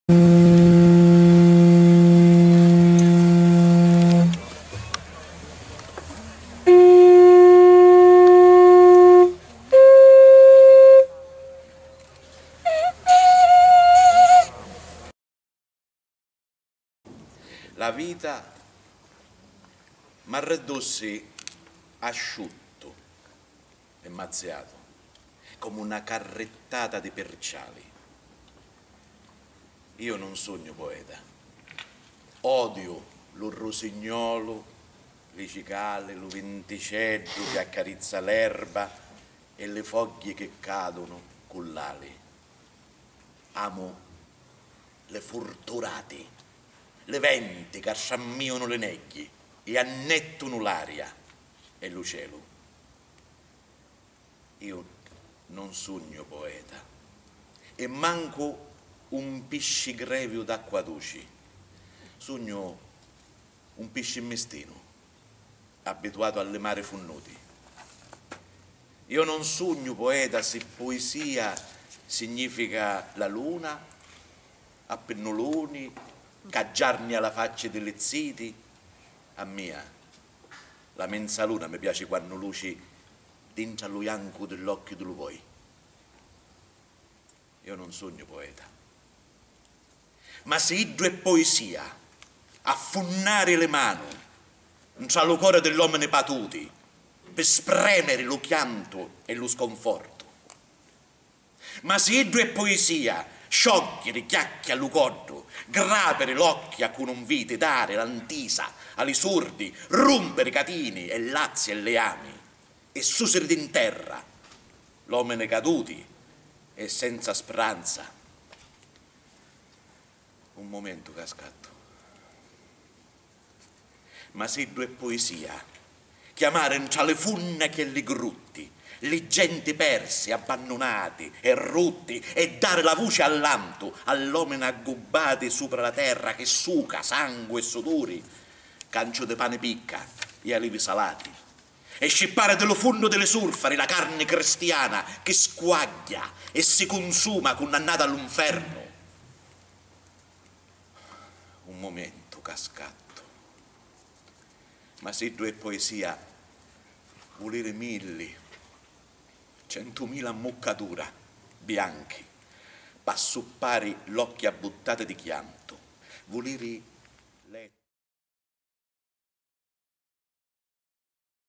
Giovanni Moschella in "Non sugnu Pueta" (Ignazio Buttitta)
Nella suggestiva cornice del chiostro di San Francesco l'omaggio ad Ignazio Buttitta con le canzoni dei Velut Luna e la voce recitante di Giovanni Moschella.

Rassegna Tindari Festival di Anna Ricciardi.
Patti. 19 Agosto 2014
Chiostro San Francesco
Riprese e montaggio video: Grazia Capone